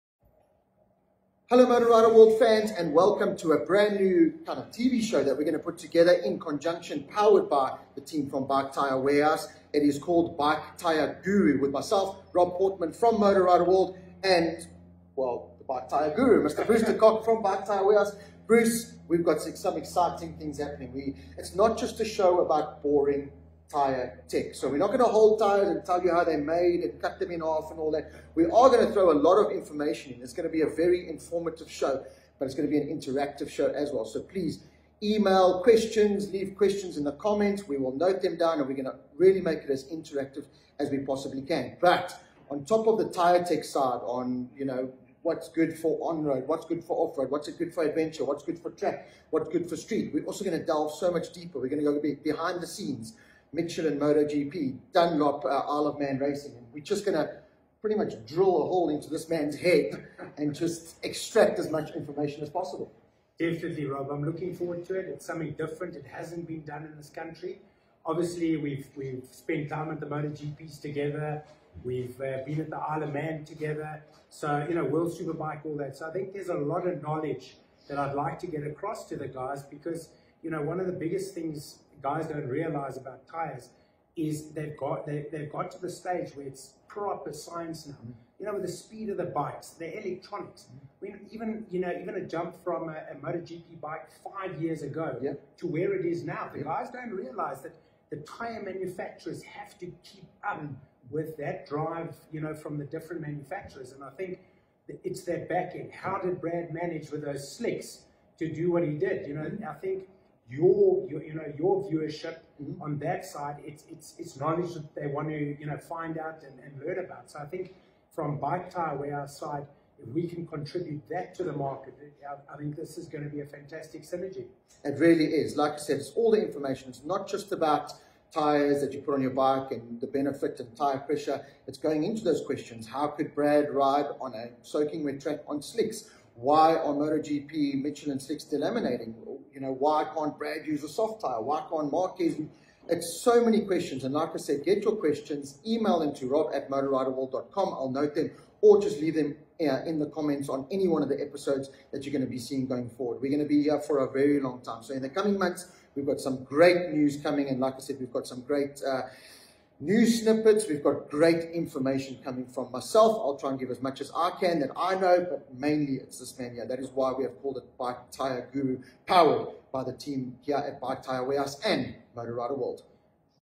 The Bike Tyre Guru - Season 01 Episode 00 Intro - Bike Tyre Warehouse and Moto Rider World
Welcome to a brand new tv show we're putting together called Bike Tyre Guru. Bruce de Kock from Bike Tyre Warehouse and Rob Portman from Moto Rider World Magazine will be hosting this new, very informative and interactive show.

This is not just a show about boring tyre tech - we will give you a lot of useful information but it will also be an interactive show.

We'll talk about what tyres are good for different conditions and we'll go behind the scenes of some of the biggest tyre manufacturers in the world. Yes, we will talk about things like Brad on the slicks on a wet track - how did he manage?!

We'll have some great news and snippets in the coming months - don't miss an episode.

Visit Bike Tyre Warehouse or the Bike Tyre Warehouse